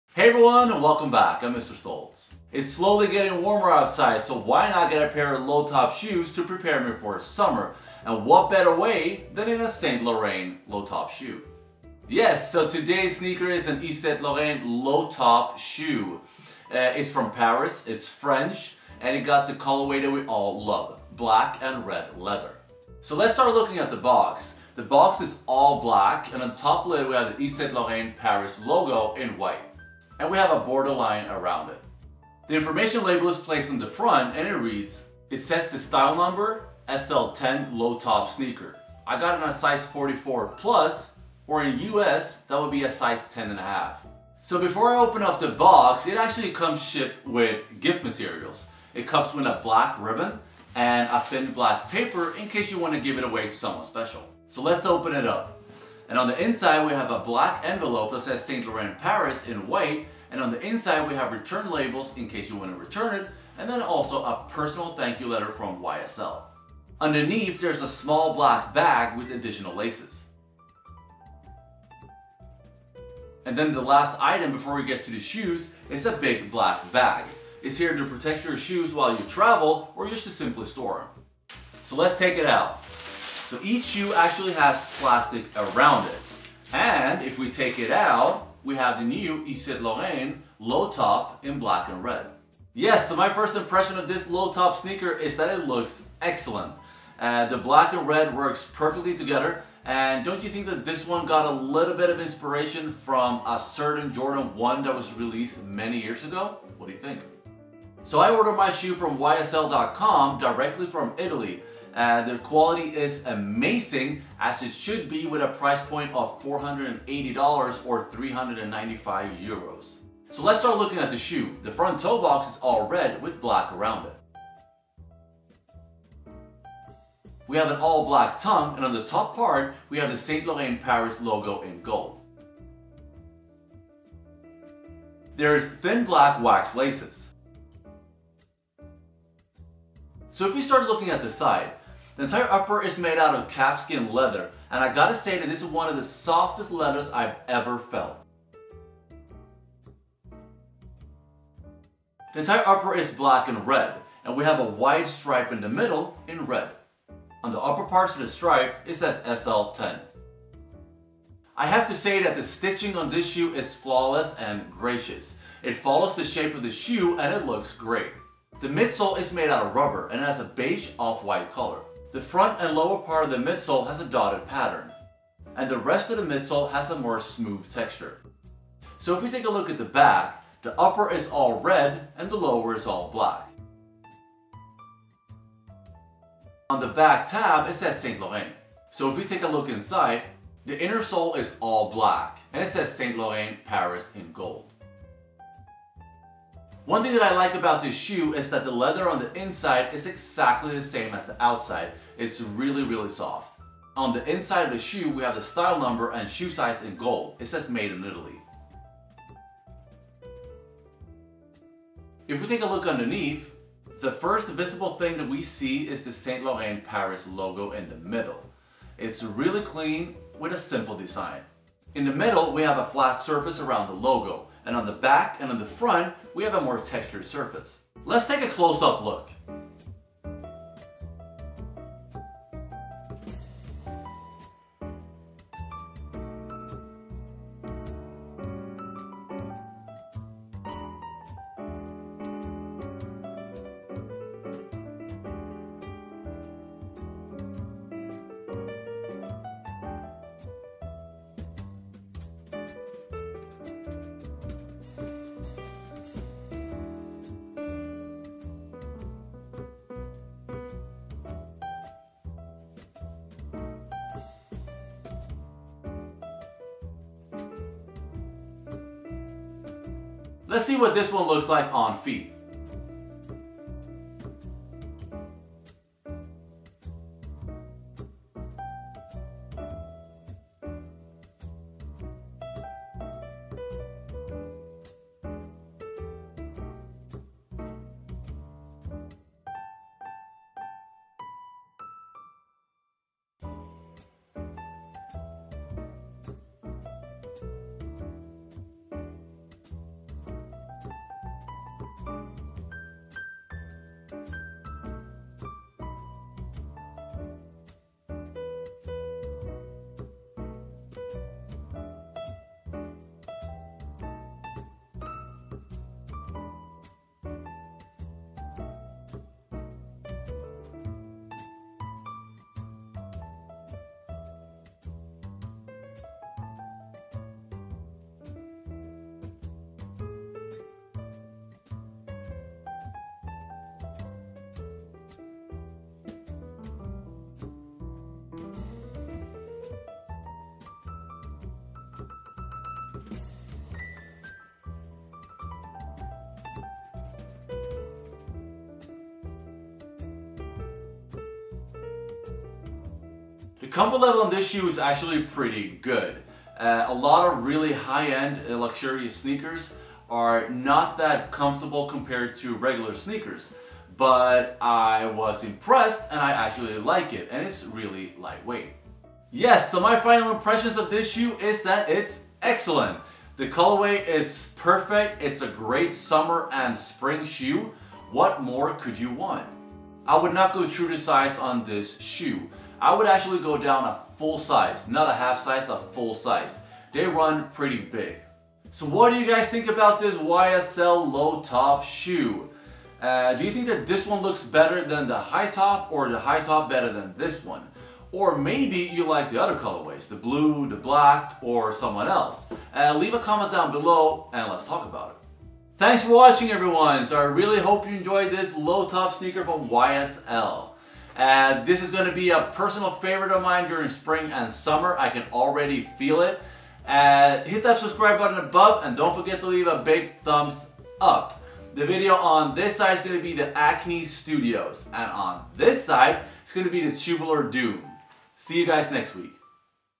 Yves Saint Laurent Court Classic Low Top SL/10 RED - Unboxing + On Feet + Close Up - Mr Stoltz 2016
Yves Saint Laurent Signature Court Classic Low Top SL/10 RED is this weeks video review and unboxing with an on-feet and close-up look.